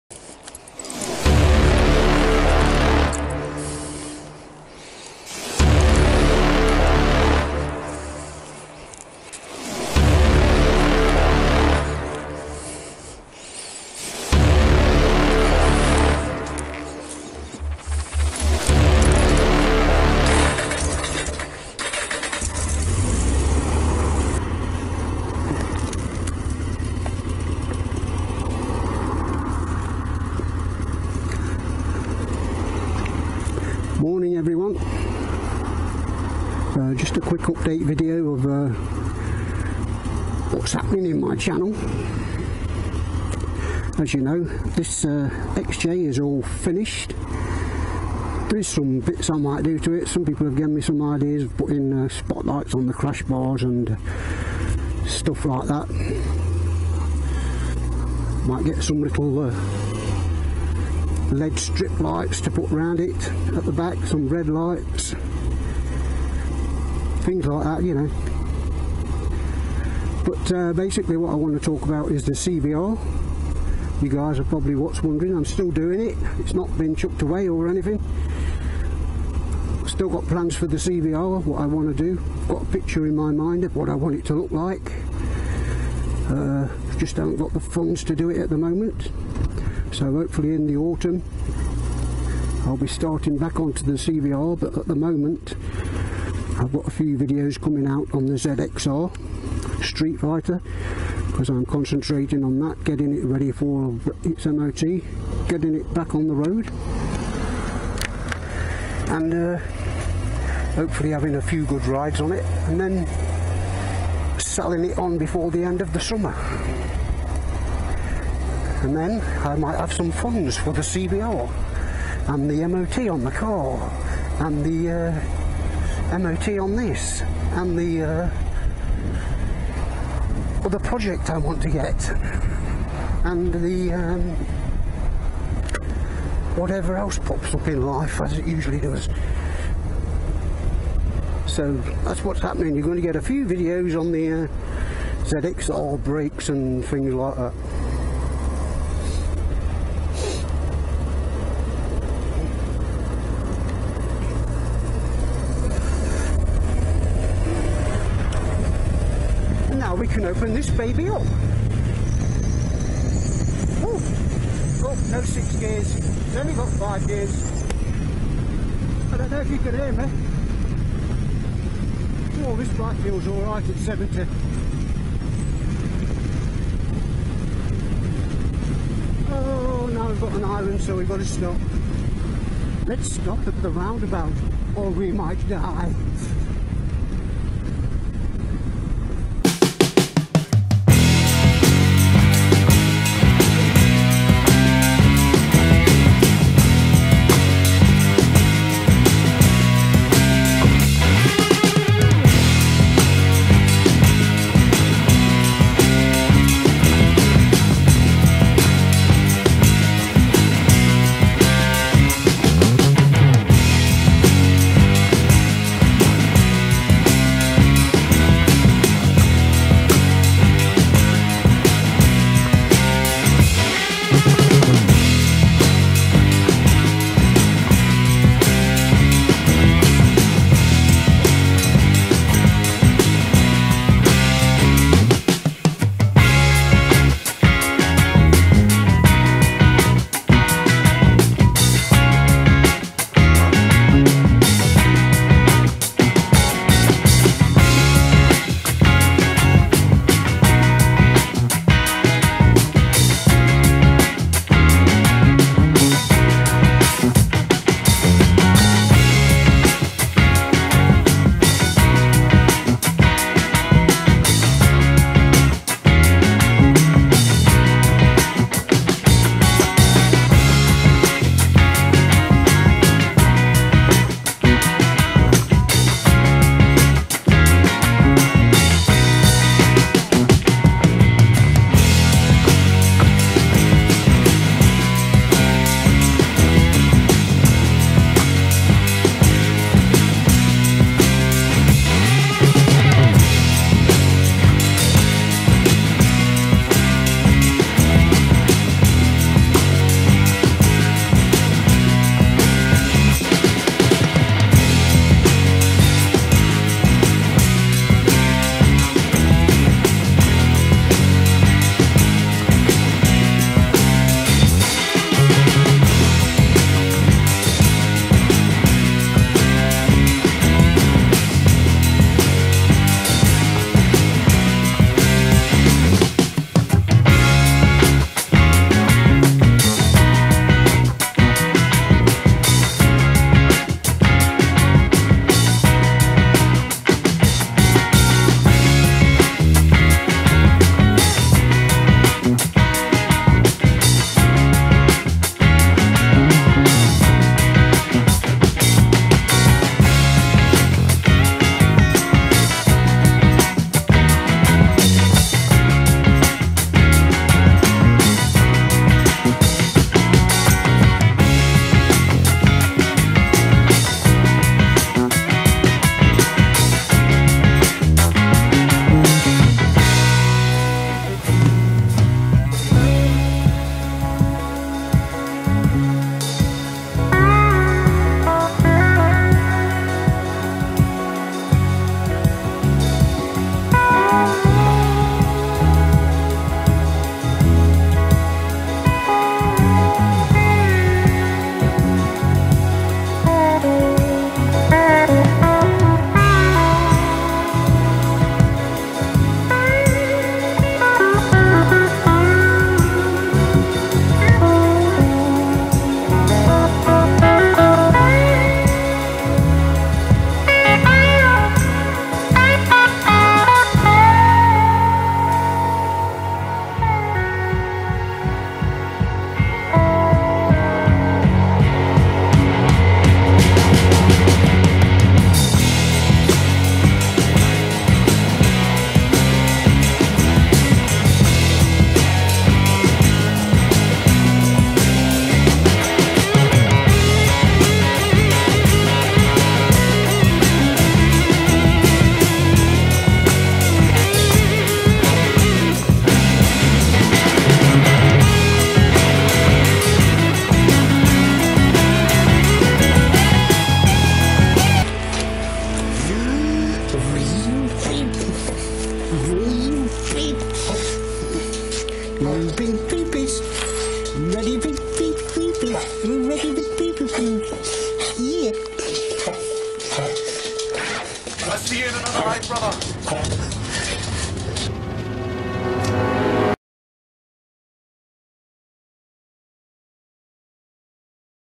dude whats happening,Channel Update.
Just letting everyone know what's going on with the cdr1000f custom project and the ZXR750H1.